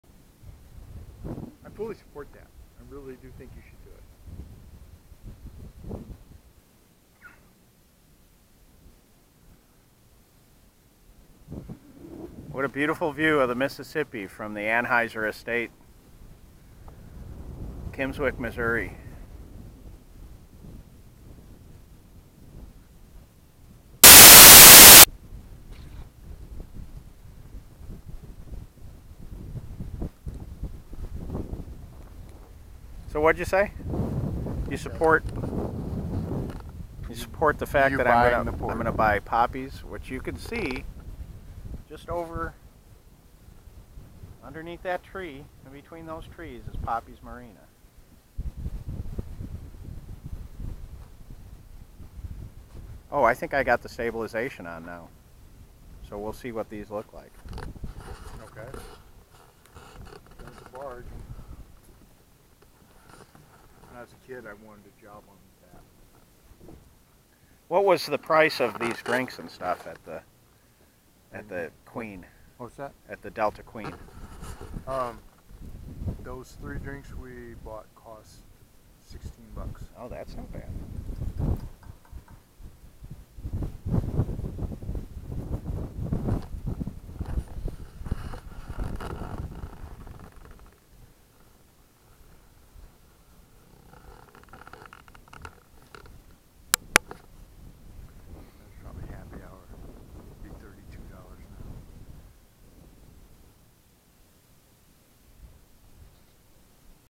Anheuser Estate at Kimmswick Missouri
View of the Mississippi River just north of Poppy's Marina at Kimmswick Missouri